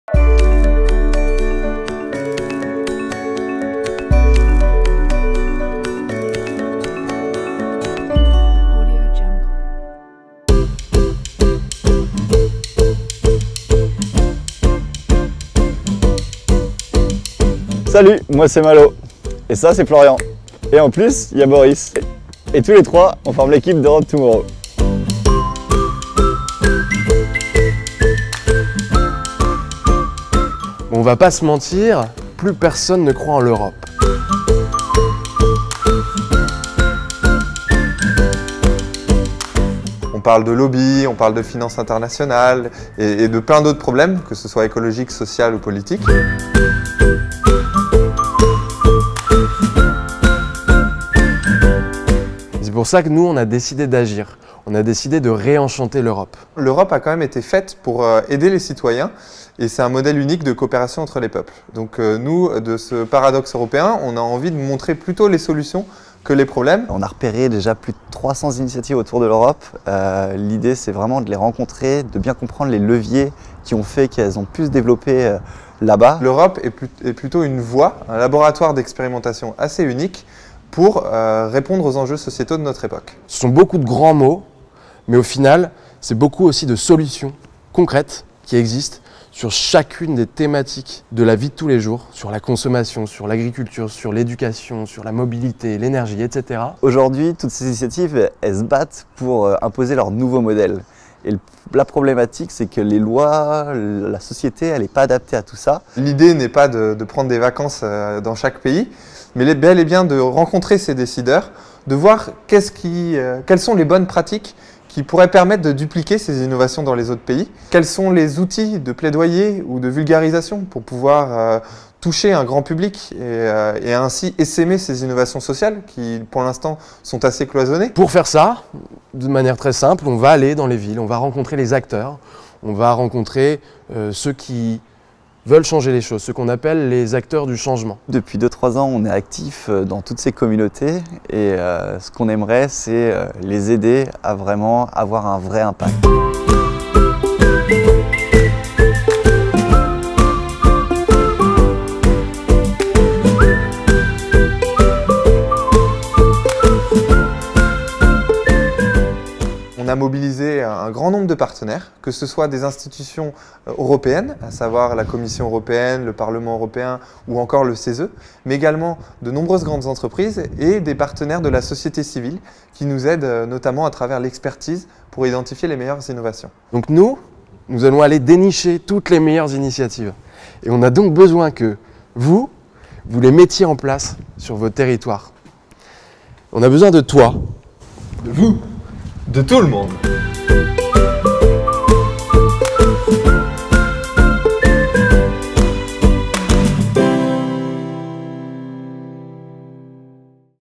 Discover Europe Tomorrow (Boris Marcel - Florian Guillaume - Malo Richard)
[FR] Découvrez l'innovation social en Europe.
Retrouvez plus d'information sur notre campagne KissKissBankBank
Projet fondé par Boris Marcel, Florian Guillaume et Malo Richard.

Modèle unique de coopération entre les peuples, société civile foisonnante, poids économique de premier ordre, berceau de la démocratie... En y regardant de plus près, l’Europe a tout pour réussir ! Elle constitue le terreau fertile qui pourrait permettre l’émergence de nouvelles solutions face à ces multiples crises. Par ailleurs, sa gouvernance continentale unique pourrait la faire devenir le laboratoire d’une véritable démocratie mondiale, écologique, juste et solidaire !

Ainsi, pour répondre aux enjeux de notre époque et parce que nous croyons en une vision positive de l’Europe, nous avons décidé d’agir !

Europe Tomorrow est un programme sur trois ans. La première année sera consacrée à identifier les innovations de terrain et à comprendre leurs "codes-source", afin d'en favoriser la duplication. Dans un second temps, nous mobiliserons des experts de chaque thématique afin d'imaginer leurs changement d'échelle et d'élaborer des scénarios prospectifs. Enfin, nous co-créerons avec les entreprises, les institutions publiques et les citoyens, des solutions qui répondent aux enjeux de notre temps.

[EN] Discover social innovation in Europe .